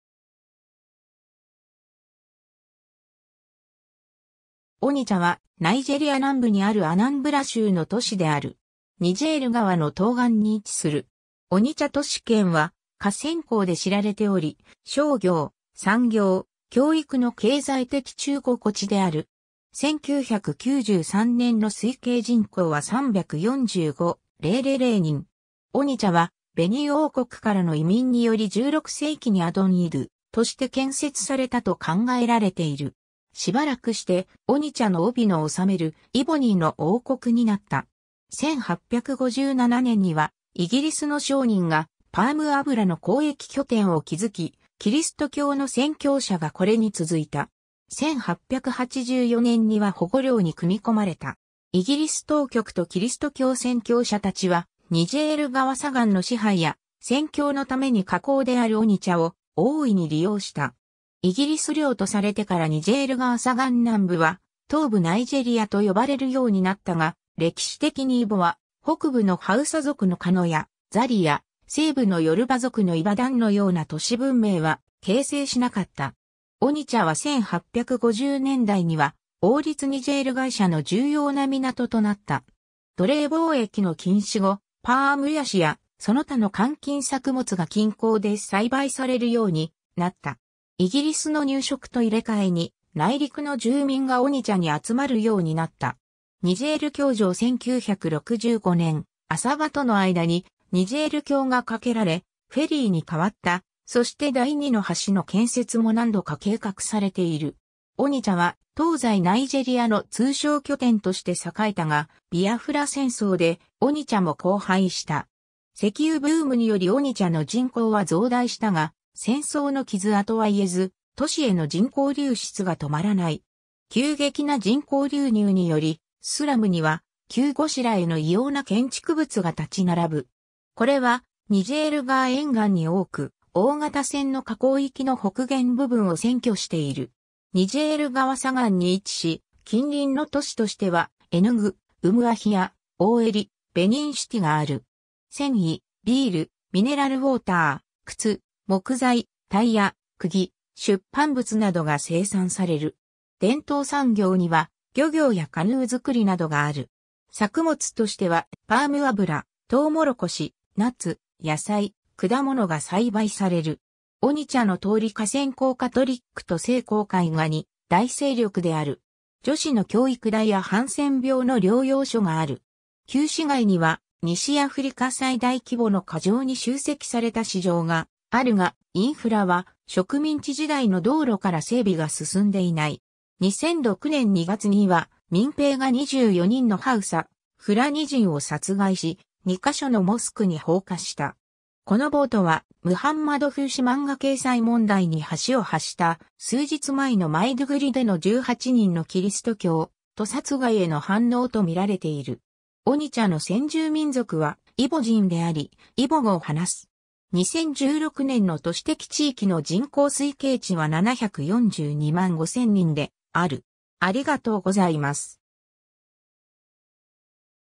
オニチャ
オニチャ（イボ語: Ọ̀nị̀chà Mmílí, 英語: Onitsha）は、ナイジェリア南部にあるアナンブラ州の都市である。
ニジェール川の東岸に位置する。
オニチャ都市圏は河川港で知られており、商業、産業、教育の経済的中心地である。
1993年の推計人口は 345,000 人。
 オニチャはベニン王国からの移民により16世紀にアド・ンイドゥ (Ado N'Idu) として建設されたと考えられている。
しばらくしてオニチャのオビ（王）の治めるイボ人の王国になった。
1857年にはイギリスの商人がパーム油の交易拠点を築き、キリスト教の宣教者がこれに続いた。
1884年には保護領に組み込まれた。
イギリス当局とキリスト教宣教者たちはニジェール川左岸の支配や宣教のために河港であるオニチャを大いに利用した。
 イギリス領とされてからニジェール川左岸南部は東部ナイジェリアと呼ばれるようになったが、歴史的にイボは北部のハウサ族のカノやザリア、西部のヨルバ族のイバダンのような都市文明は形成しなかった。
オニチャは1850年代には王立ニジェール会社の重要な港となった。
奴隷貿易の禁止後、パームヤシやその他の換金作物が近郊で栽培されるようになった。
イギリスの入植と入れ換えに内陸の住民がオニチャに集まるようになった。
 ニジェール橋上 1965年アサバとの間にニジェール橋が架けられフェリーに代わったそして第二の橋の建設も何度か計画されている。
 オニチャは東西ナイジェリアの通商拠点として栄えたが、ビアフラ戦争でオニチャも荒廃した。
石油ブームによりオニチャの人口は増大したが、戦争の傷跡は癒えず、都市への人口流出が停まらない。
急激な人口流入によりスラムには急拵えの異様な建築物が立ち並ぶ。
これはニジェール川沿岸に多く、大型船の可航域の北限部分を占拠している。
 ニジェール川左岸に位置し、近隣の都市としてはエヌグ、ウムアヒア、オウェリ、ベニンシティがある。
 繊維、ビール、ミネラルウォーター、靴、木材、タイヤ、釘、出版物などが生産される。
伝統産業には漁業やカヌー造りなどがある。
作物としてはパーム油、トウモロコシ、ナッツ、野菜、果物が栽培される。
 オニチャの通り 河川港 カトリックと聖公会が2大勢力である。
女子の教育大やハンセン病の療養所がある。
旧市街には西アフリカ最大規模の過剰に集積された市場があるが、インフラは植民地時代の道路から整備が進んでいない。
 2006年2月には民兵が24人のハウサ、フラニ人（ムスリム）を殺害し、2ヶ所のモスクに放火した。
この暴徒はムハンマド風刺漫画掲載問題に端を発した数日前のマイドゥグリでの18人のキリスト教徒殺害への反応と見られている。
 オニチャの先住民族はイボ人であり、イボ語を話す。
2016年の都市的地域の人口推計値は7,425,000人である。